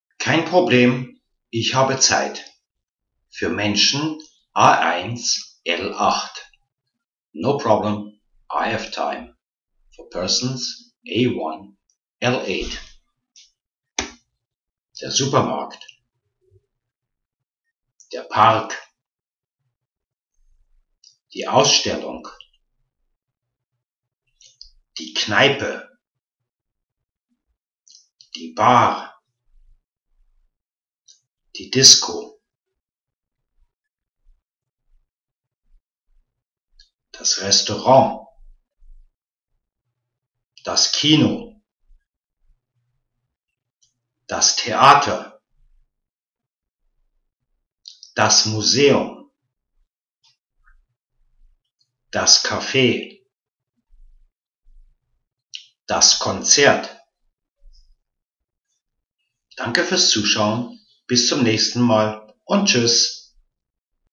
Platze : M版A1L8常見公共場所(後續: 動詞 gehen in用法) - 比恩語文
我們相信: 在5G的世代，學對外語可以提升人生! 此影片由德籍老師錄音，讓您一開始就學對德語! 您想快速通過德檢嗎? 點選連結 或